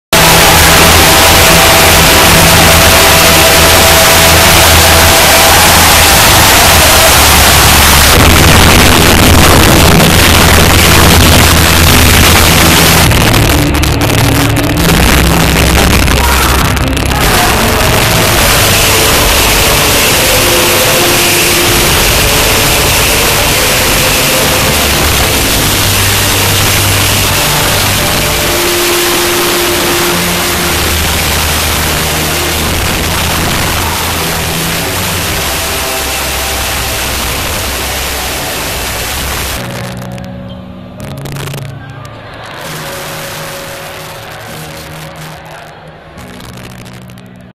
Shaolin Kung Fu show amazes Fijians
'Chinese Kung Fu, Stylish Shaolin' show amazed spectators in Viti Levu, capital of Fiji.